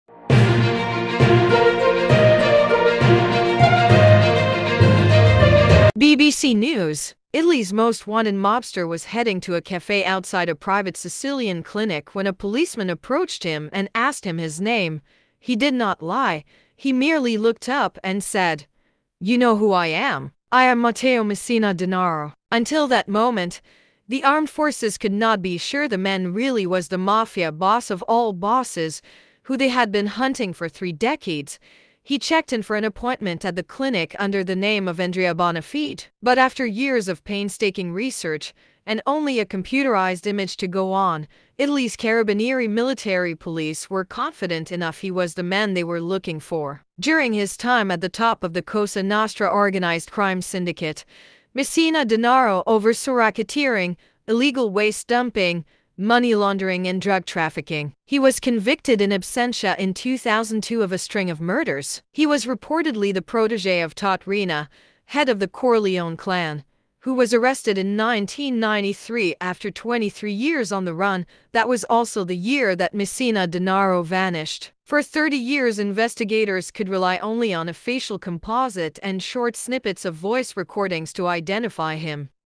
Matteo Messina Denaro: How Mafia boss was caught on a visit to a clinic
Italy's most wanted mobster was heading to a cafe outside a private Sicilian clinic when a policeman approached him and asked him his name.

He did not lie.

He merely looked up and said: "You know who I am. I am Matteo Messina Denaro."

Until that moment, the armed forces could not be sure the man really was the Mafia "boss of all bosses", who they had been hunting for three decades.

He checked in for an appointment at the clinic under the name of Andrea Bonafede.

But after years of painstaking research, and only a computerised image to go on, Italy's Carabinieri military police were confident enough he was the man they were looking for.

During his time at the top of the Cosa Nostra organised crime syndicate, Messina Denaro oversaw racketeering, illegal waste dumping, money-laundering and drug-trafficking. He was convicted in absentia in 2002 of a string of murders.